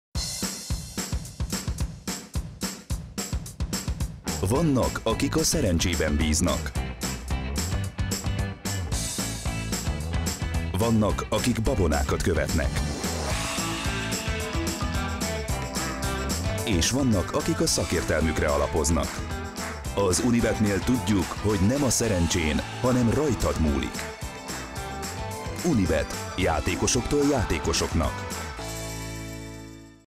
It's Not Luck, It's You | Unibet
We were lucky to win an international pitch of Unibet and to develop their TV campaign for Hungary, Russia and the Baltic countries. Entirely at random, we decided to upload here the Hungarian TV spot.
Was it luck?
Was our decision unprejudiced?